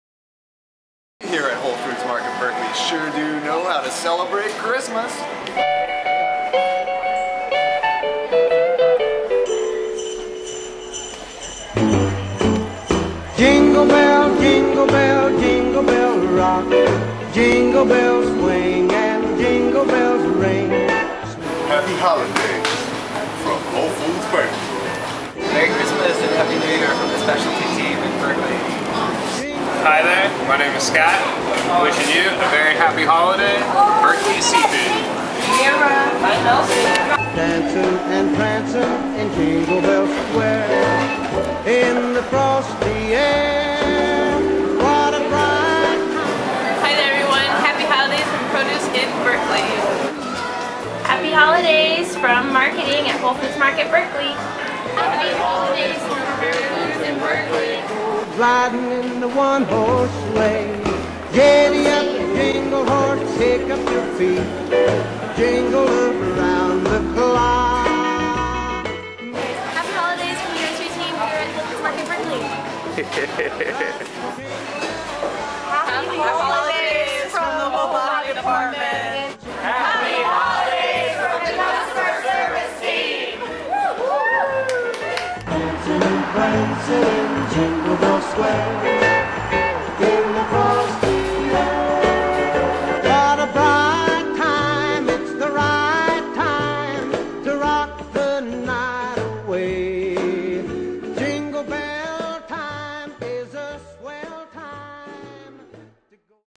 Happy Holidays from Whole Foods Market Berkeley
Wishing you a warm and happy holiday full of amazing food, decadent drinks, and the people you love! From all the team members here at Whole Foods Market Berkeley